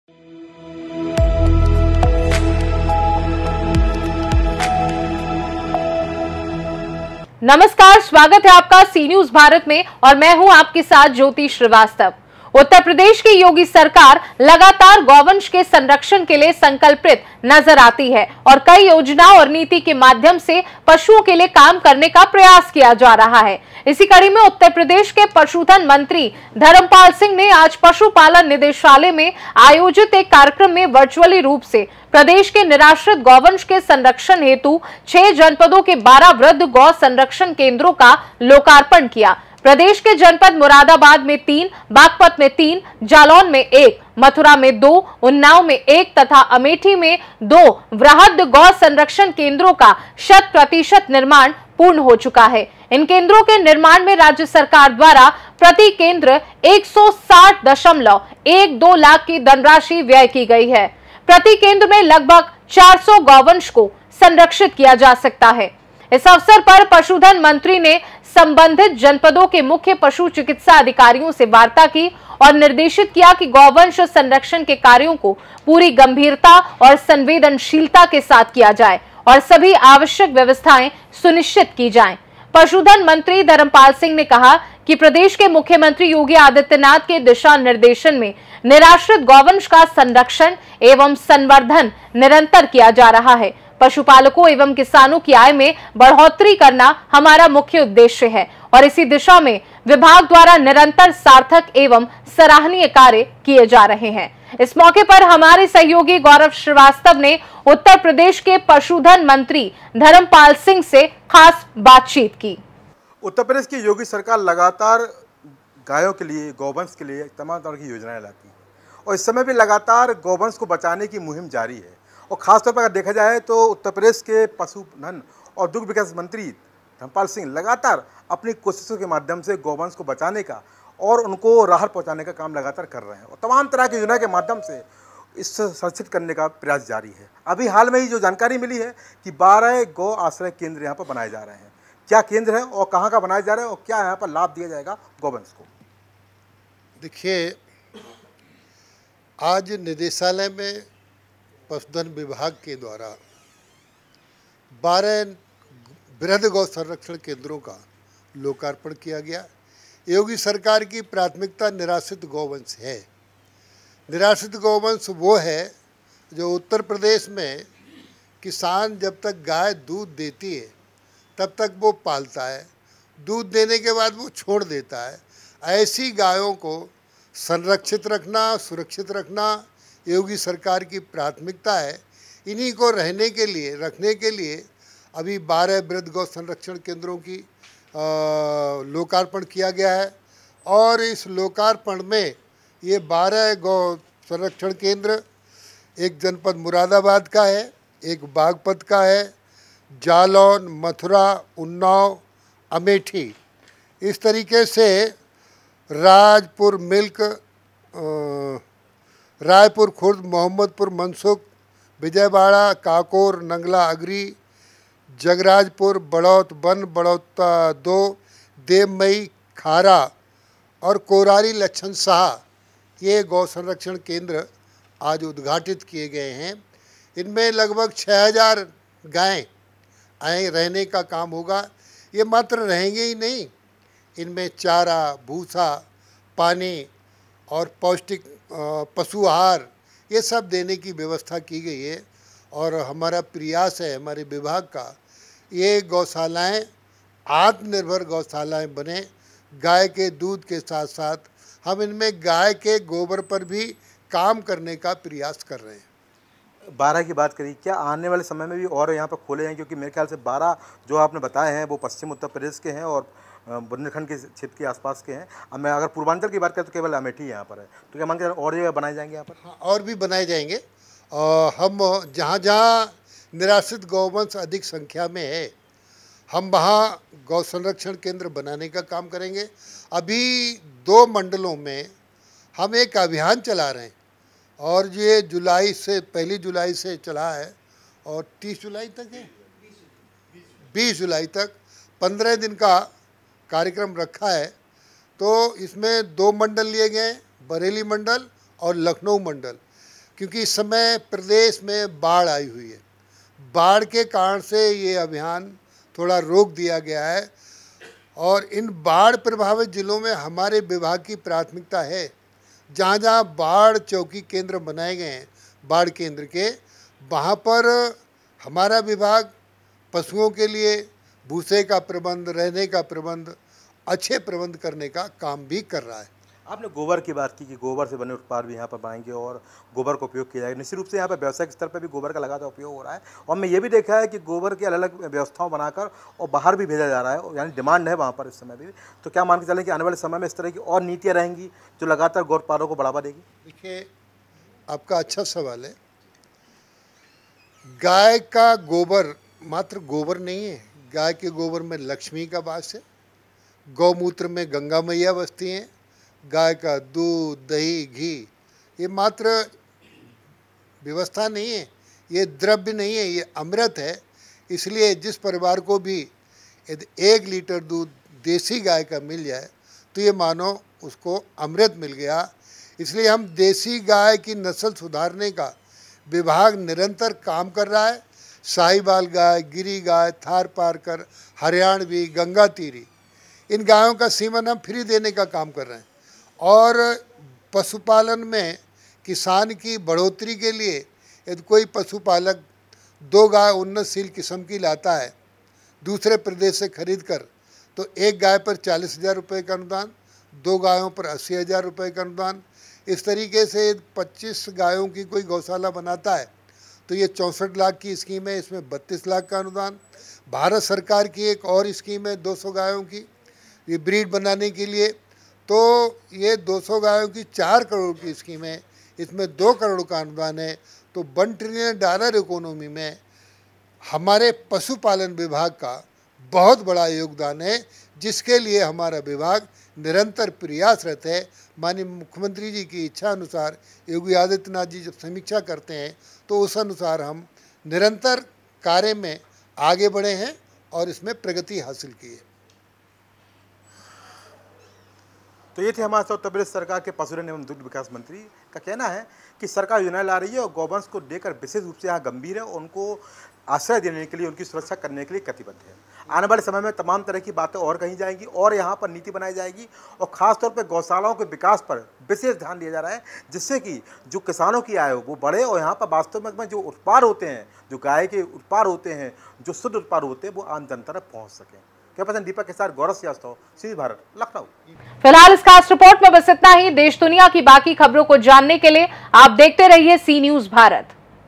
12 वृहद गो संरक्षण केन्द्रों का उत्तर प्रदेश के पशुधन मंत्री ने किया लोकार्पण || Cnews Bharat
12 वृहद गो संरक्षण केन्द्रों का उत्तर प्रदेश के पशुधन मंत्री ने किया लोकार्पण || Cnews Bharat